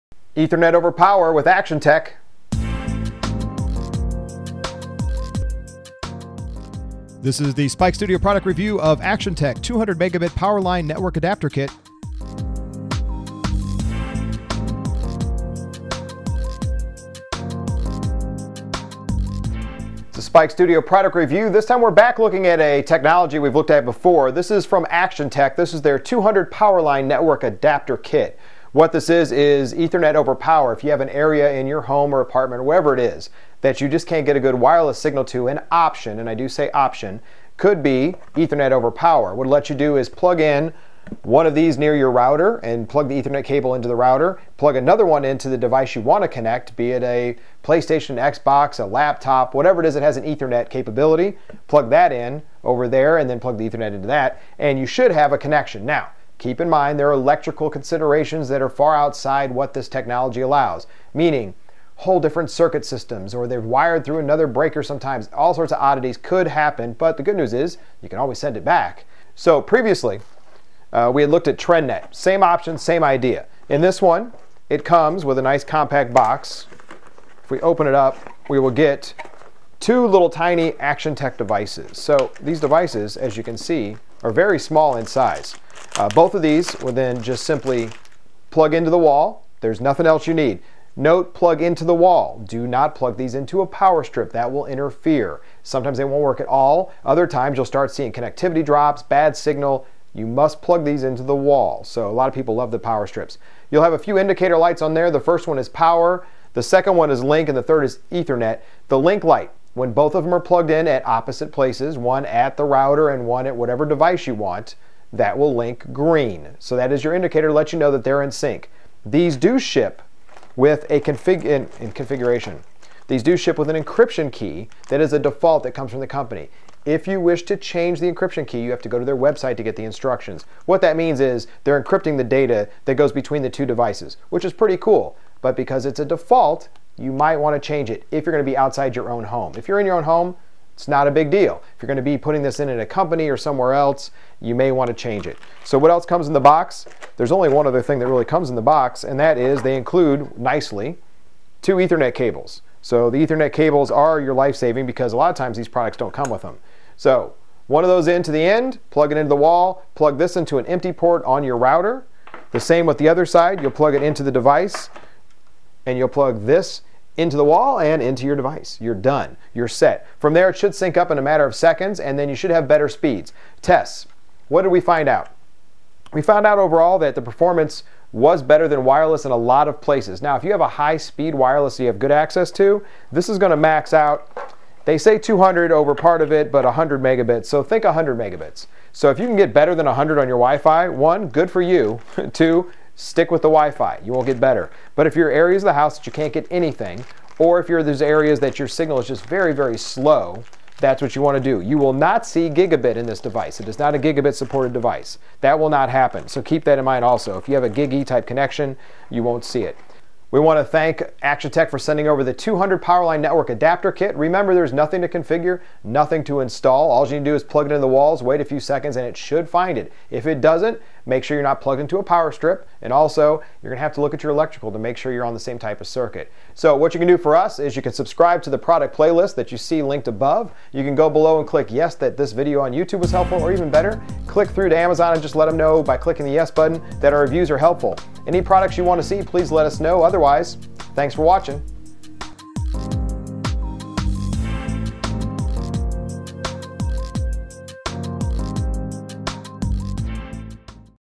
Actiontec 200 Mbps Powerline Network Adapter Kit PWR200K01 review
We have used and tested products similar to Actiontec 200 Mbps Powerline Network Adapter and brought one along for a work site, just in case. We were happy we did. There was no wifi and no network access to be found in the temporary office in another country. The walls would not let the wifi through. Luckily we found that the entire place was on the same electrical and we tried this out. Success!

We plugged one Actiontec 200 Mbps Powerline Network Adapter next to the router (into the plug as the directions and may other reviewers say) and one next to an access point. Using the ethernet over power we basically made an access point available for the few of us to use. It was excellent.the signal strength seems great and tests showed we were getting the same ISP speed as those in the other side connected to the router directly.

It took a second for the light to go green on the Actiontec 200 Mbps Powerline Network Adapter letting us know it was in sync, so we were worried. But we found that the same test again later gave the same result. It just took a few moment for it to find the other one on the electrical system.

I never noticed overheating or warmth from the device. It just worked. See the video for more and get your hands on it